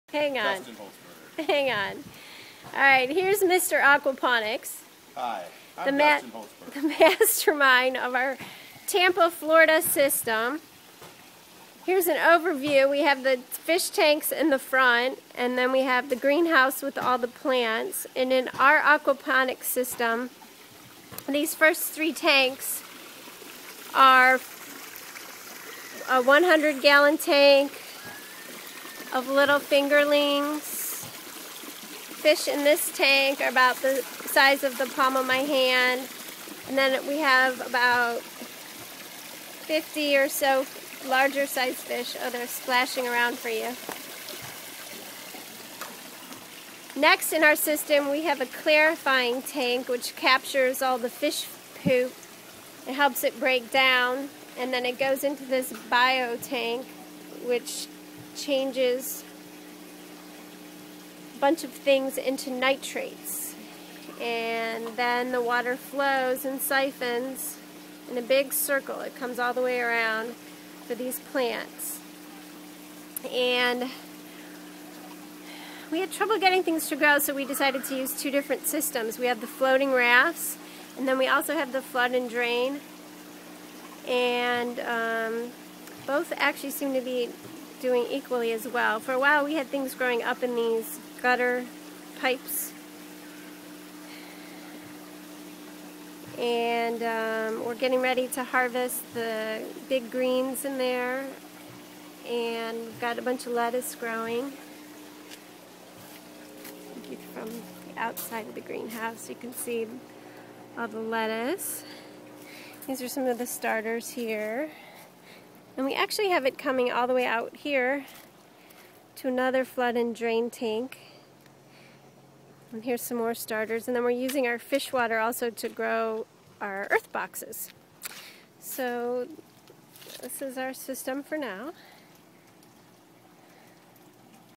Lutz, FL Side Yard Aquaponics System
Our side yard system has 500 gallons of fish (about 150 total Tilapia), 100 gallon clarifying tank, 100 gallon bio tank, and about 800 gallons of plant tanks with two nice size flood and drain tanks.